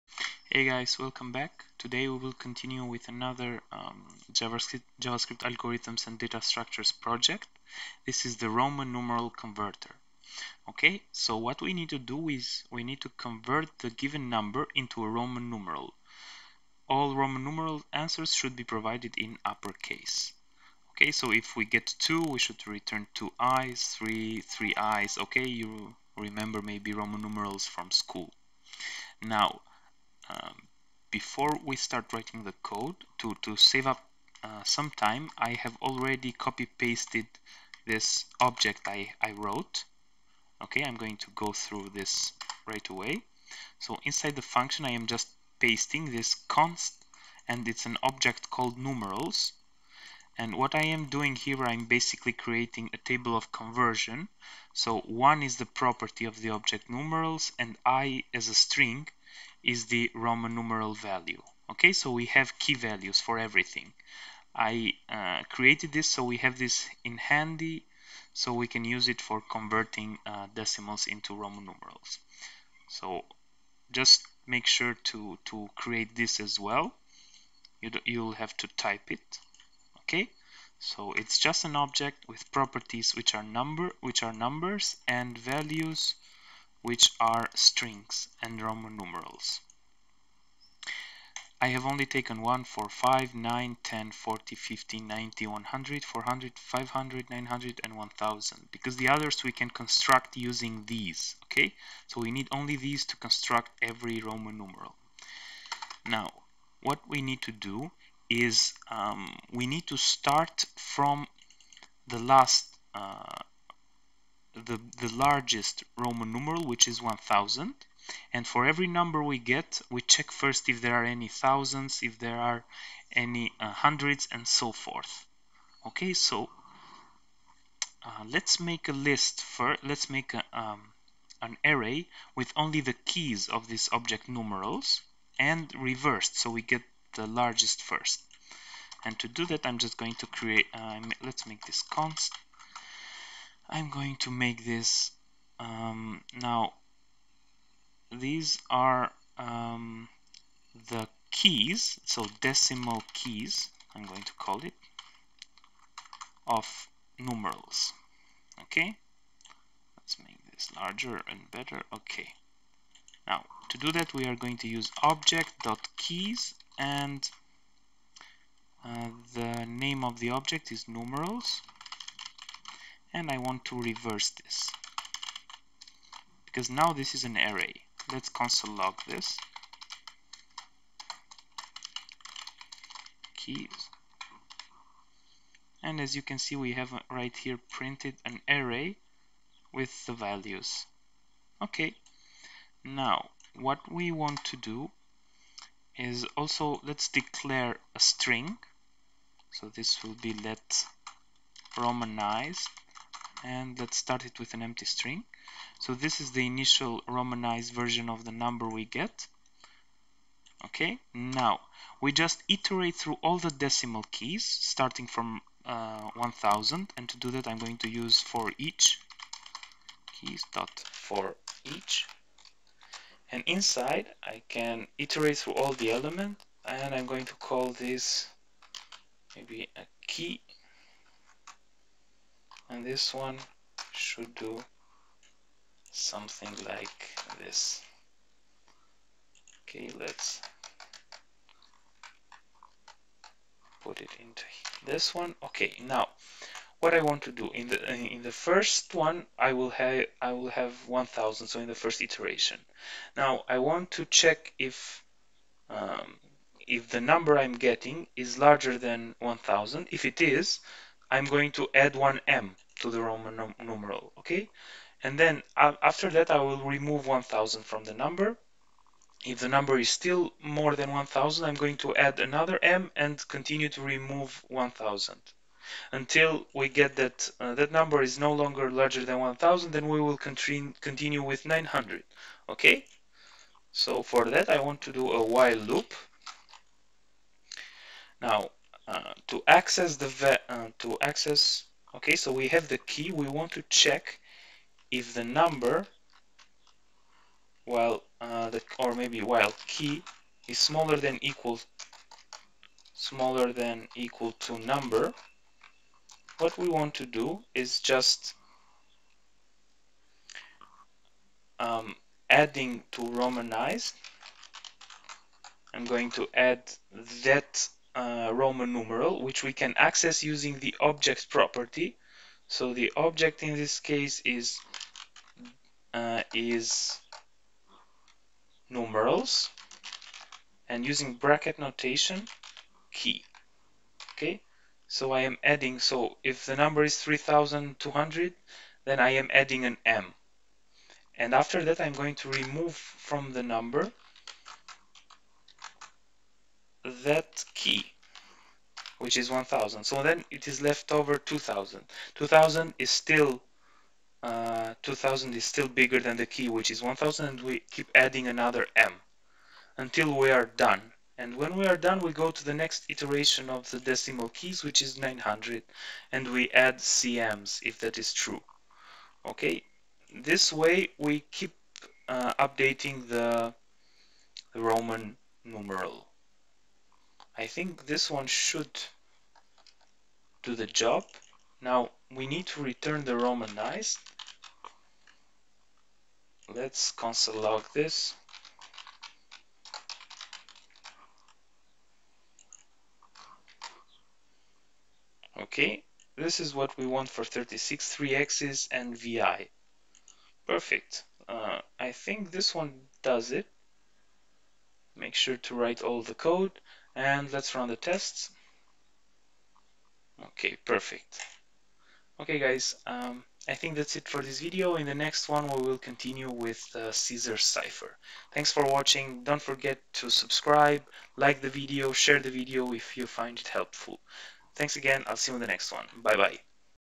freeCodeCamp solutions - Roman Numeral Converter
In this video series I am going to go through the exercises in the Freecodecamp's curriculum, for JavaScript Algorithms and Data Structures: